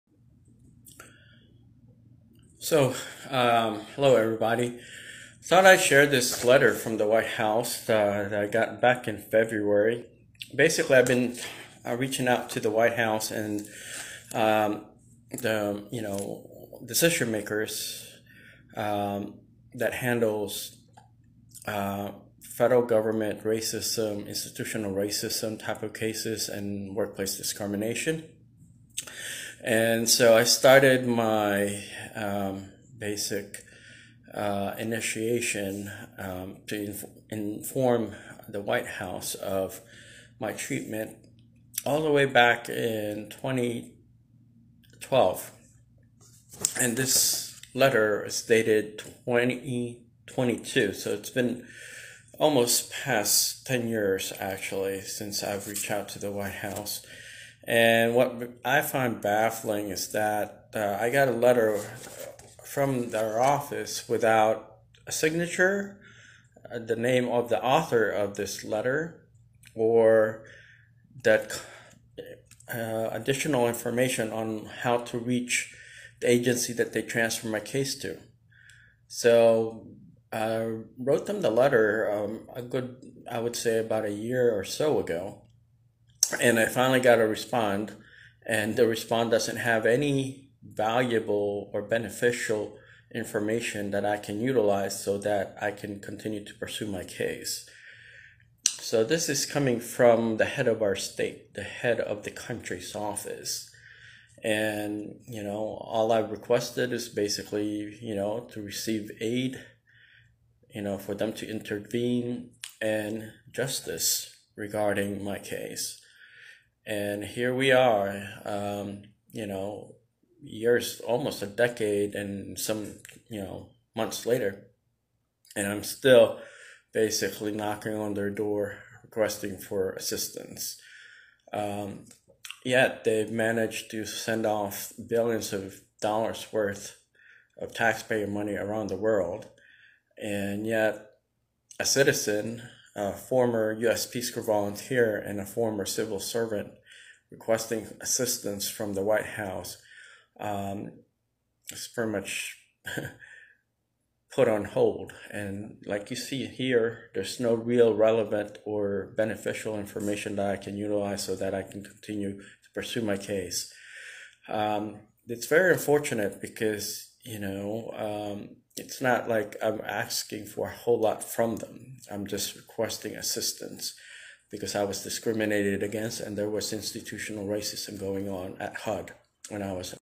White House Letter
Biden-Harris Administration.

Al was blamed for the bureaucracy and procedural issues that they've created.

Brave Heart Message

This is a story about ONE versus MANY, a nobody, standing up to a powerful Washington DC establishment, and seeking justice to be served. A man who has gone through the ODDS and kept on moving mountains with just ONE voice. An American, by choice who refuse to let HATE win the future even, if he is standing all alone.
Aldous Mina, is the son of legal immigrants, the family gave up everything to immigrate to America, in search of a better future and opportunity to give back. Both parents took on menial jobs in order to support three children….. Aldous, was the middle child. The family lived in poverty below the average income for the same size family in the US, in Virginia.
Aldous, went through all the odds in order to commit to his GOAL of obtaining an education and be able to contribute to society. He was active in the local community serving the less fortunate members of the Lynchburg, Virginia immigrant society. Aldous was also a volunteer at a local church and various other community service organizations.
Upon graduation, Aldous, decided to volunteer in the US Peace Corps in Romania, where he worked with various student organizations, NGO’s, local business organizations, marginalized communities and economic development institutions. Al was not paid a regular salary, but was given a stipend to live and work in the country for twenty-eight months. He endured all sort of challenges, but persevered in his task and community commitment.
Aldous was not given the same opportunity to succeed as the other new hires within the department who happens to come from an African background from the Caribbean and Continental Africa. But rather, he was the target by the staff of African descent. Aldous endured racism and discrimination for the whole duration of his employment with HUD, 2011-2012. His civil and human rights violated by the US government and is requesting aid, relief and justice to be served.
(Aldous Mina, decided to go forward with his story in order to help save the next generation of Americans from the debilitating effect of racism. He is also determined to curb the effects of racism to the US economy, and specifically to save the US tax payers millions of dollars being wasted in US individual earning potential and economic (GDP) growth as an effect of institutional racism and discrimination in the workplace. Aldous, is NOT out there to blame a specific race or point a finger at a group of Americans……WE ARE ALL IN THIS TOGETHER and he wants his story to help heal America and catapult our country to an economic and social beacon for others to follow).
YEARS OF FIGHTING FOR A BETTER AMERICAN FUTURE FOR ALL AMERICANS~